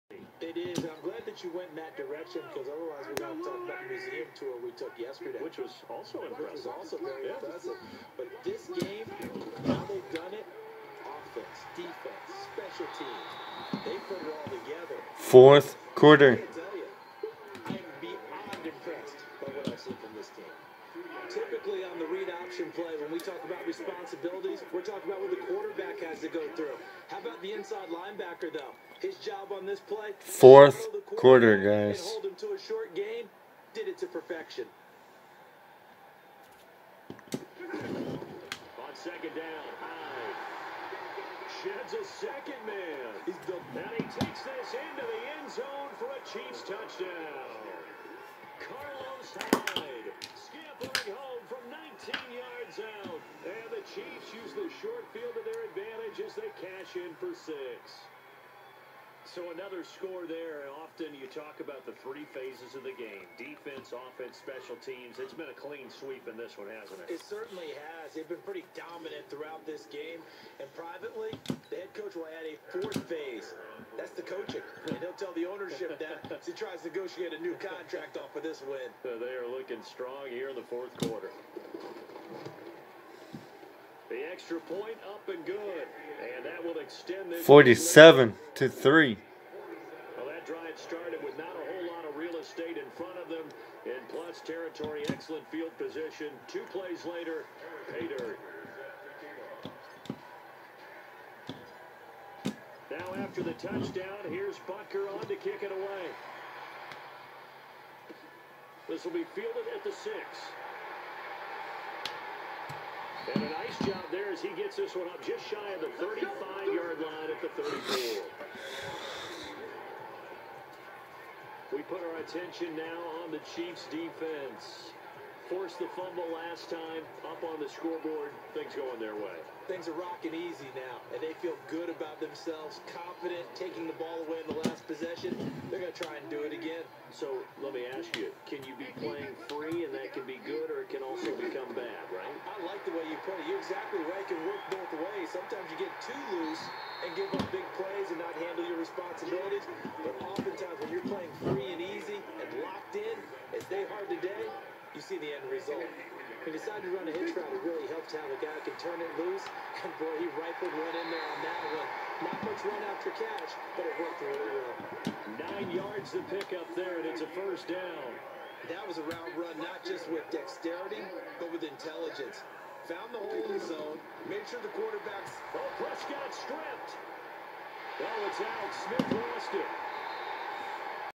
Kansas City Chiefs vs Dallas Cowboys ( 4th Quarter )
Chiefs vs Cowboys
4th Quarter
47-3 Is The Score For Now
I will see you Guys on The Next Game!